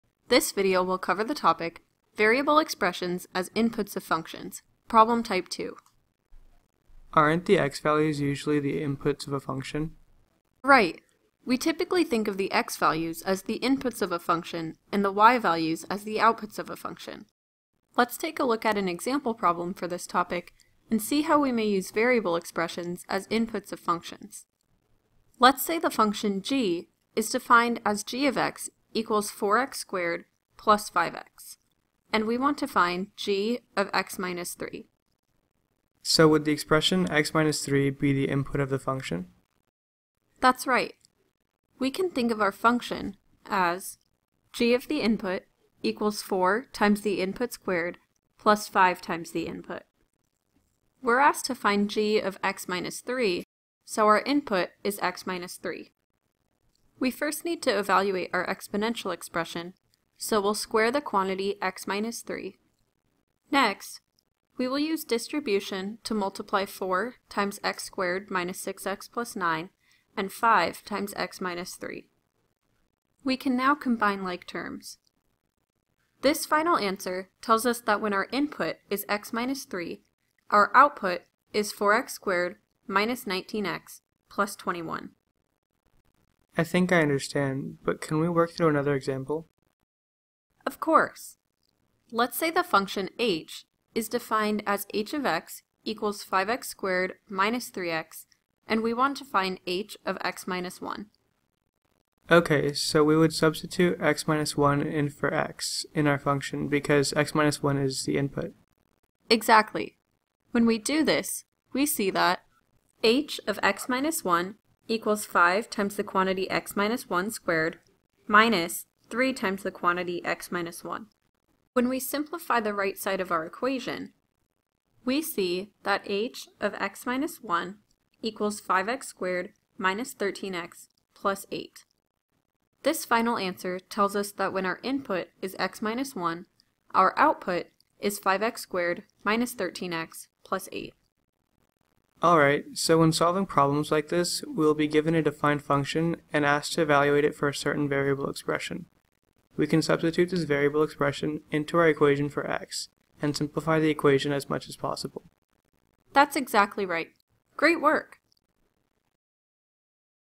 Topic: Variable Expressions as Inputs of Functions: Problem Type 2
This is a topic level video of Variable Expressions as Inputs of Functions: Problem Type 2 for ASU.

Join us!